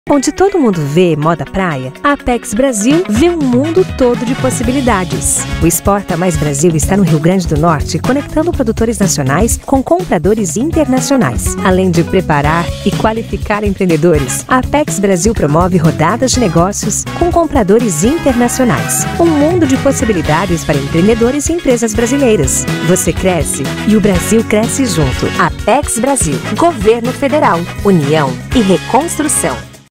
O Rio Grande do Norte está ganhando o mundo! | Produtor
Onde todo mundo vê exportação, a gente vê geração de empregos, renda e uma economia mais forte para o Rio Grande do Norte.

O Exporta Mais Brasil é o programa da ApexBrasil que conecta produtores regionais com compradores internacionais. Clique e saiba mais!